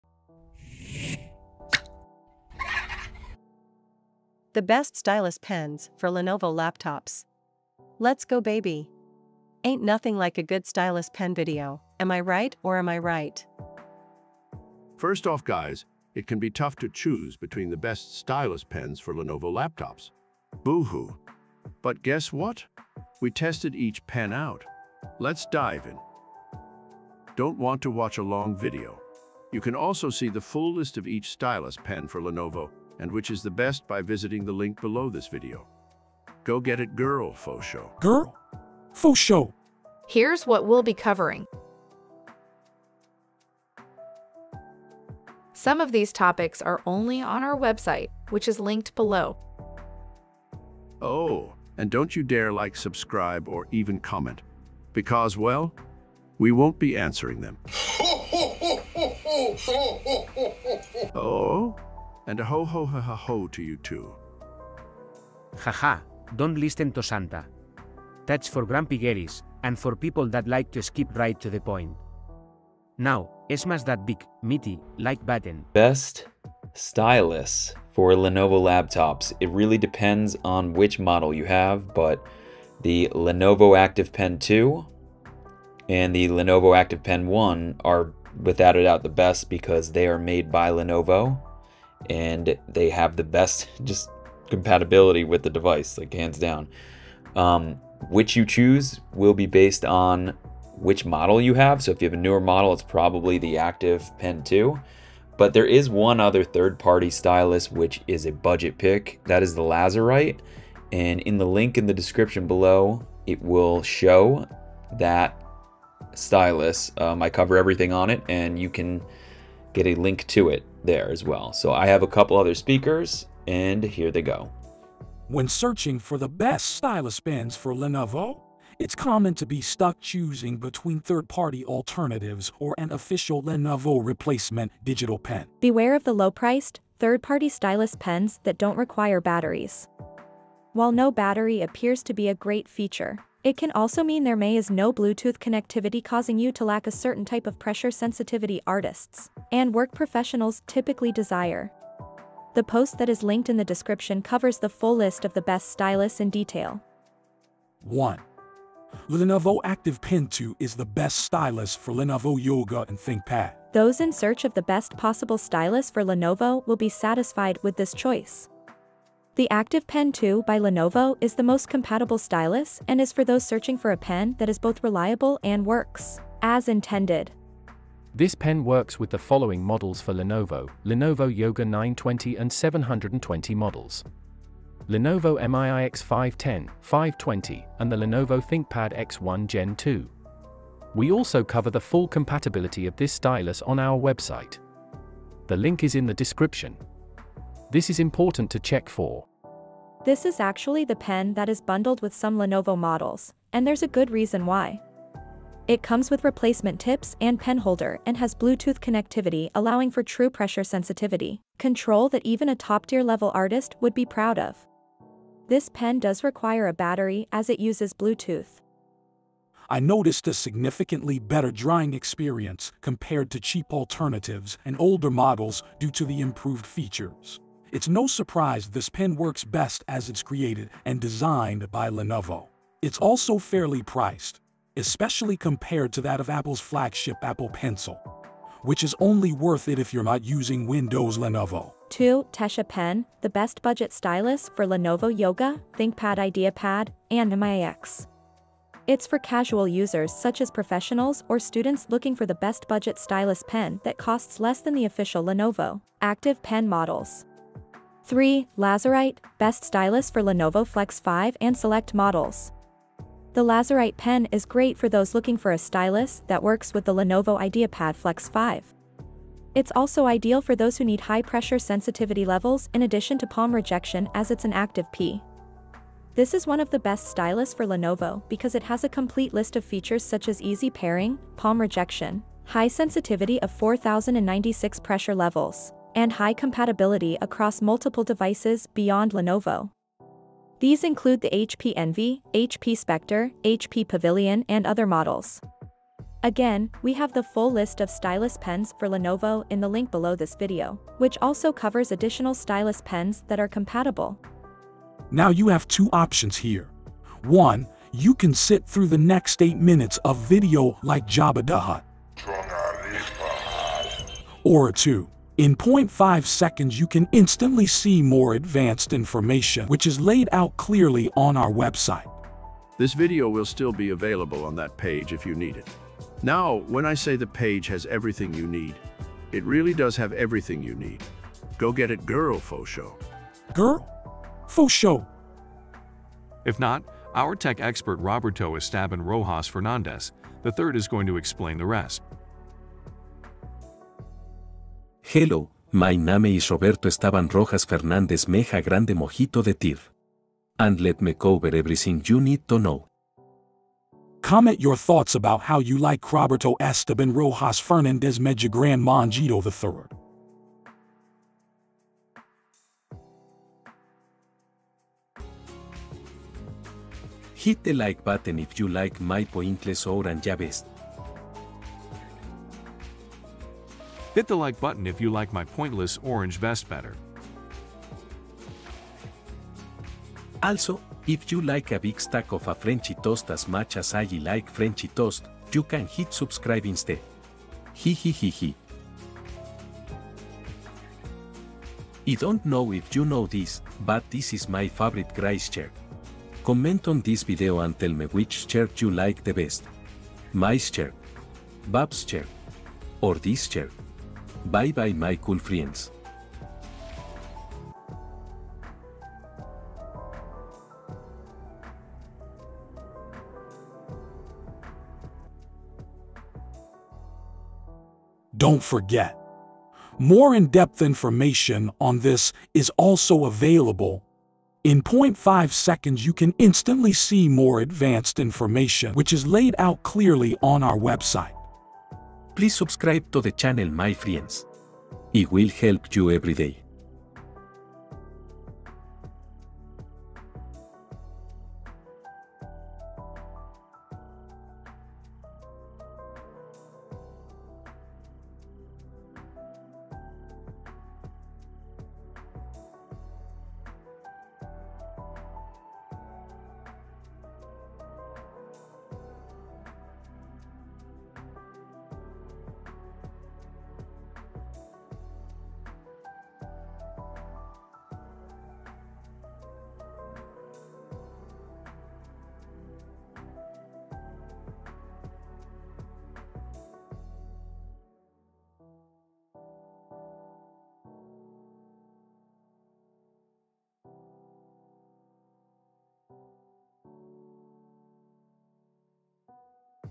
I tested the 7 Best Stylus Pens for Lenovo Yoga, ThinkPad, and IdeaPad
What are the best Best Stylus Pens for the Lenovo Yoga, ThinkPad, IdeaPad, and other Lenovo Laptops?

There are often third party alternatives to Lenovo's official replacement digital pens when searching for the best stylus pens for Lenovo. The purpose of this video is to discuss the pros and cons of using inexpensive pens versus Lenovo's active pen 1 and 2.

00:00 Intro
01:20 Top 3 Pens I tested and which is the best - Quick Glance
02:02 Each Pen explained Individually
02:43 1. Lenovo Active Pen 2 (Best Overall)
04:05 2. Tesha Pen
04:23 3. Lazarite Stylus
05:11 Two Important Options
05:39 What our tech experts say
06:07 Quick Summary of everything
06:59 Conclusion

Almost all of the Lenovo pens listed in this post are active pens, which allow you to draw lines in response to pressure.